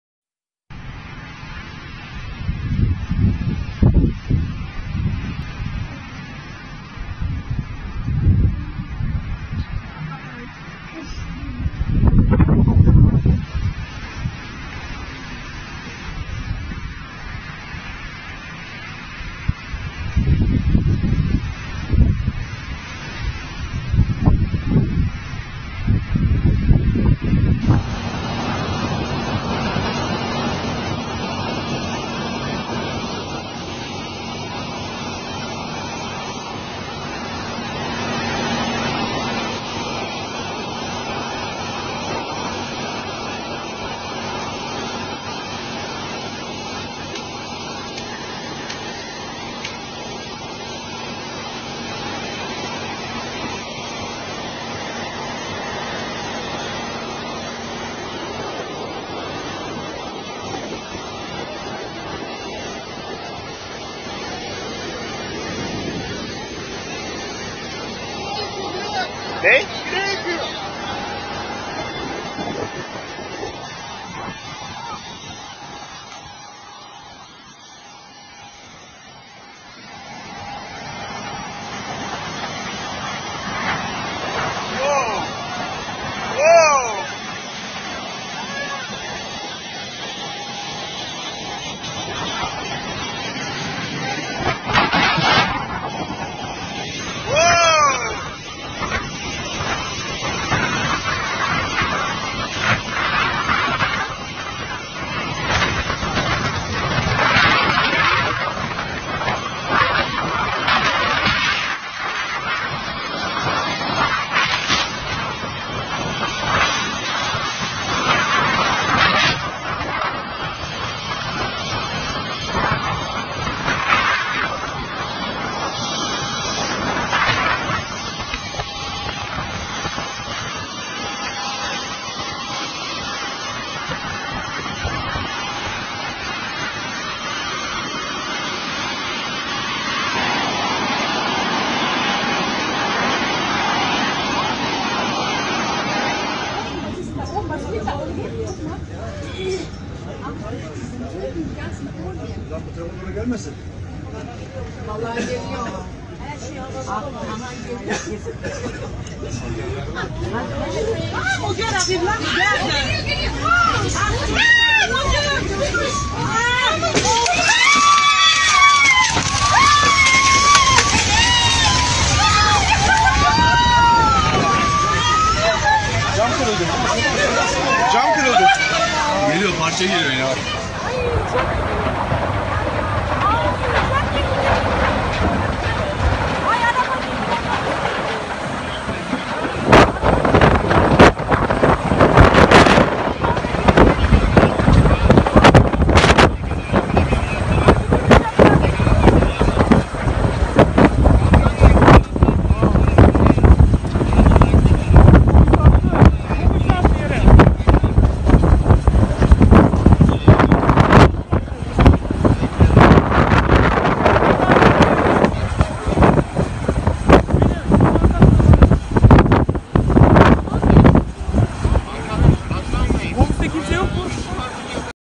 Tornado golpea aeropuerto de Antalya en Turquía
El saldo que arrojó hasta ahora el furioso embate de un tornado sobre el aeropuerto de Antalya es de 12 heridos, ómnibus volcados, escaleras destruidas y varias aeronaves dañadas.